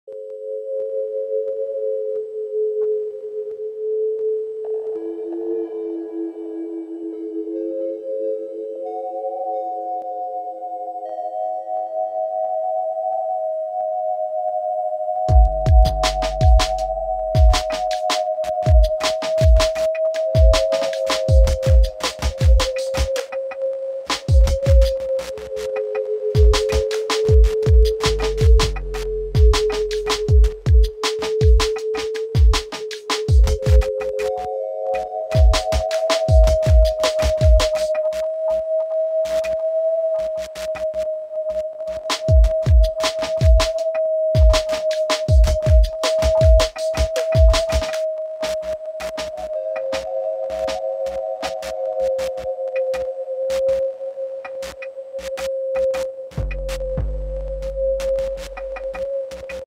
"Sessions 6"
excerpt from a series of sketches using a broken cell phone, vintage signal generator, and loop sequencer.